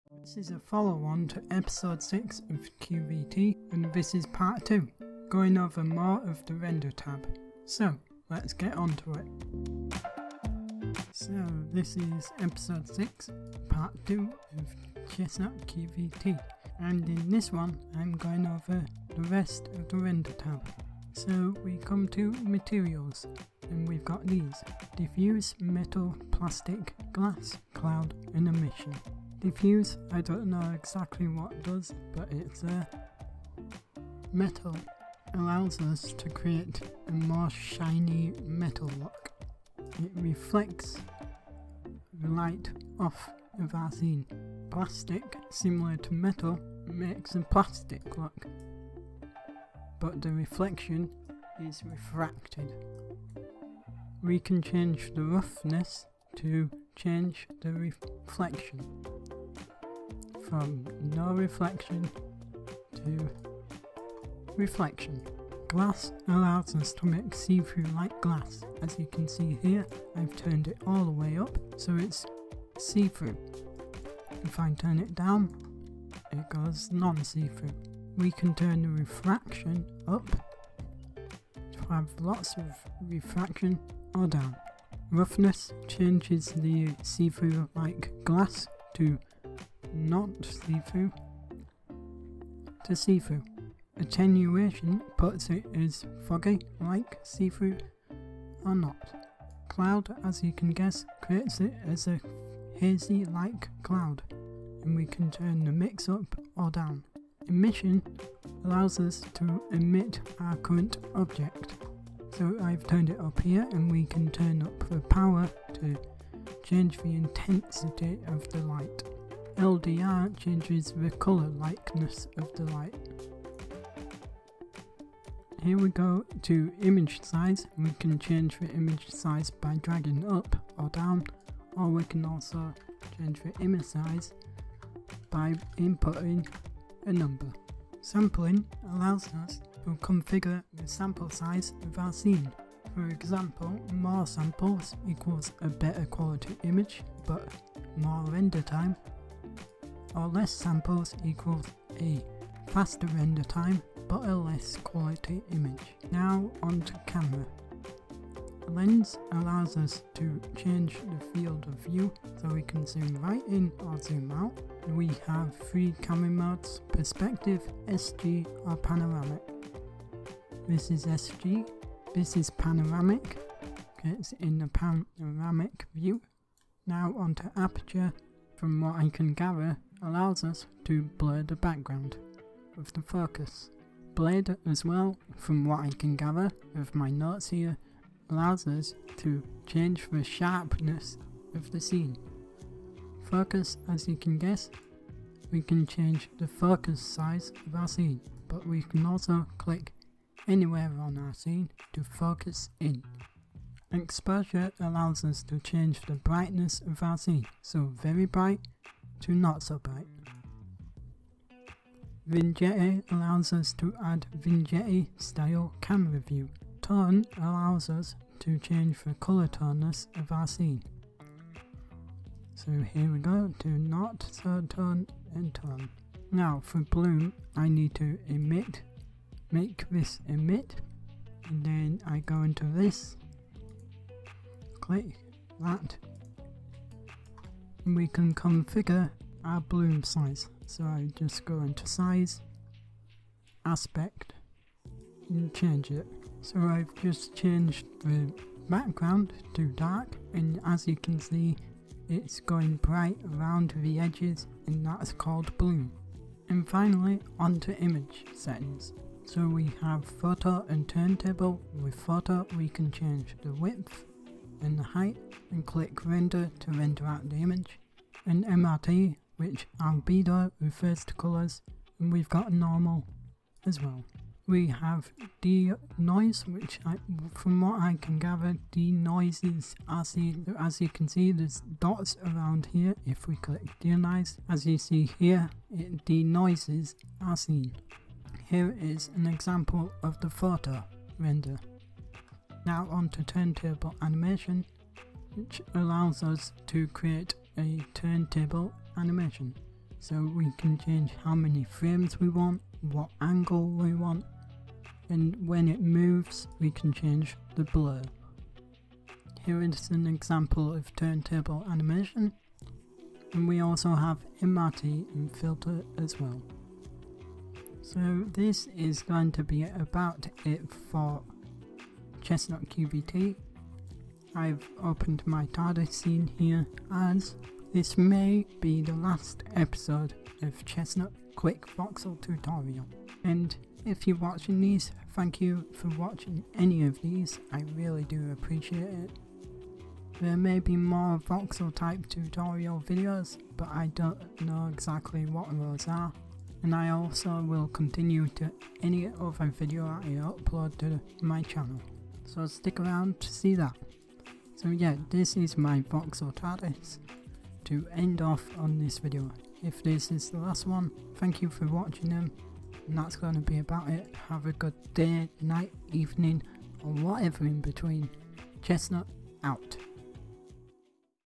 Chestnut QVT Ep6 Part2 Rendertab extras
This is Part2 of the 6th episode of Chestnut QVT and in this one I go over more of the Render tab!

simple step
Timeless